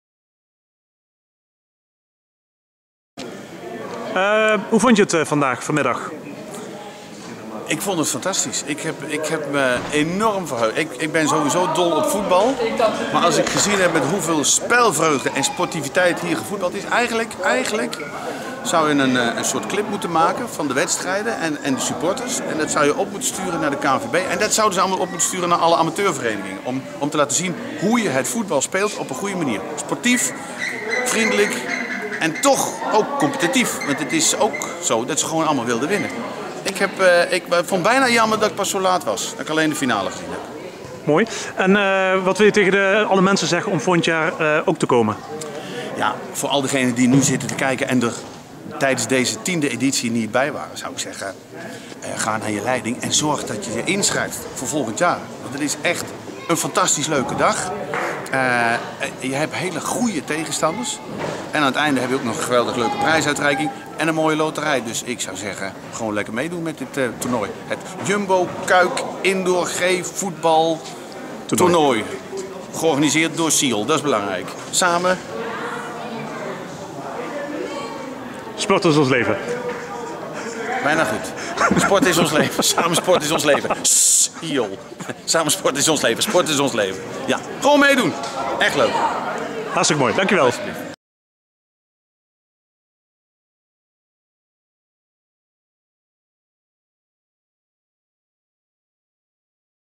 Jumbo Cuijk Indoor G Toernooi; Jochem van Gelder
Jochem van Gelder was te gast bij de Jumbo Cuijk Indoor G-Toernooi 2017. Bekijk wat hij er van vond en waarom je er volgend jaar bij moet zijn!